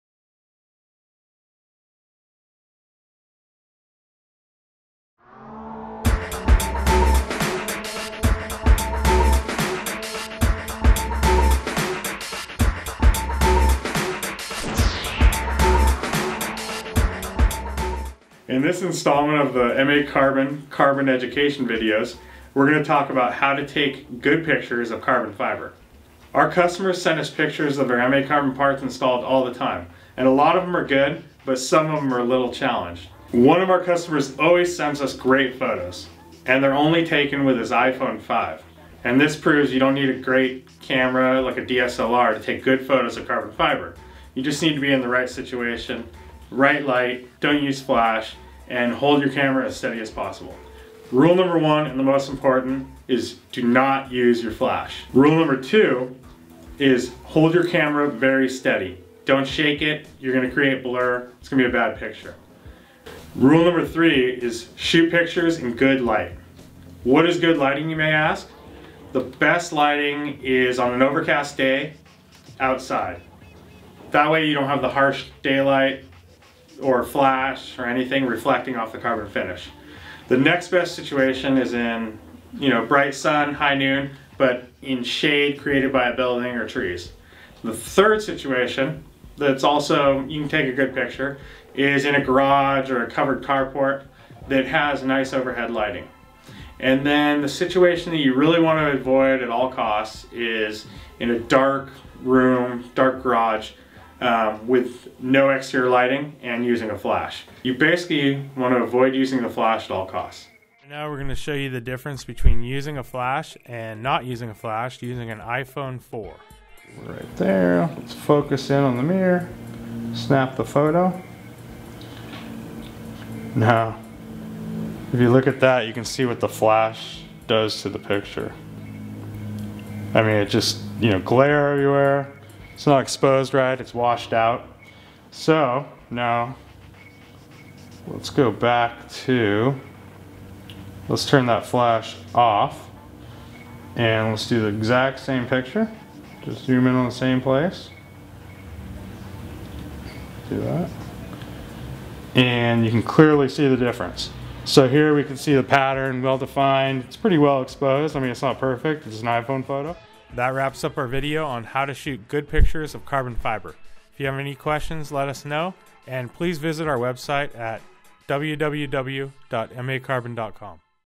How-To Photograph Carbon Fiber (MAcarbon Carbon-Education)
In this MAcarbon, Carbon-Education video we give a few pointers on how to take pictures of carbon fiber parts.